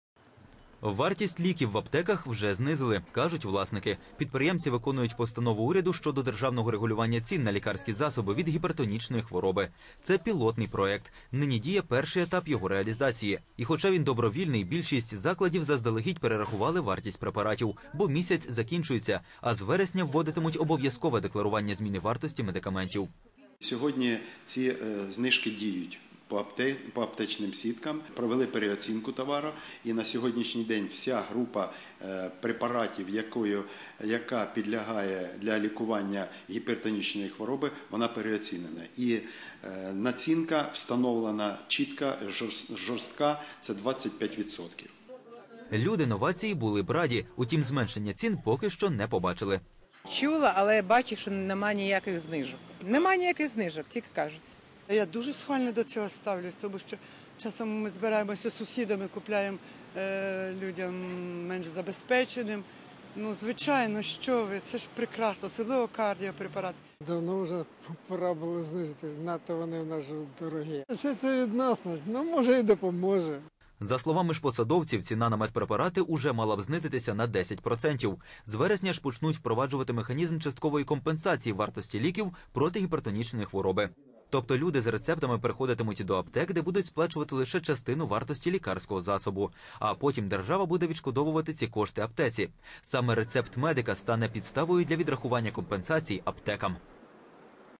Ліки від тиску подешевшали
ubr.ua - украинский бизнес ресурс : Дешеві ліки в аптеках з'явилися. Ідеться про кардіопрепарати.&nbsp; Кажуть фармацевти. Це наслідок дії постанови уряду зі зниження вартості соціально важливих медикаментів. Нині у&nbsp; списку 270 назв, які підпадають під програму. Із них 243 - проти гіпертонії. Решта - найпопулярніші на ринку.&nbsp;
Власть запретила рекламу безрецептурных лекарств (Список)
Вартість ліків в аптеках вже знизили. Кажуть власники. Підприємці виконують постанову уряду щодо державного регулювання цін на лікАрські засоби від гіпертонічної хвороби. Це пілотний проект. Нині діє перший етап його реалізації. І хоча він добровільний, більшість закладів заздалегідь перерахували вартість препаратів. Бо місяць закінчується. А з вересня вводитимуть обов'язкове декларування зміни вартості медикаментів.
Валерій Гудзенко, генеральний директор мережі аптек: &quot;Сьогодні ці знижки діють по аптечним сіткам. Провели переоцінку товару і на сьогодні вся група препаратів для лікування гіпертонічної хвороби, вона переоцінена. І націнка встановлена чітка, жорстка - це 25%&quot;.
В аптеках Киева появились бесплатные лекарства
Люди новації були б раді. Утім зменшення цін поки що не побачили.
Обіцяні дешеві ліки гіпертоніки зможуть купити не скрізь
За словами ж&nbsp; посадовців, ціна на медпрепарати уже мала б знизитися на 10 процентів. З вересня ж почнуть впроваджувати механізм часткової компенсації вартості ліків проти гіпертонічної хвороби. Тобто, люди з рецептами приходитимуть до аптек, де будуть сплачувати лише частину вартості лікарського засобу. А потім держава буде відшкодовувати ці кошти аптеці. Саме рецепт медика стане підставою для відрахування компенсацій аптекам. .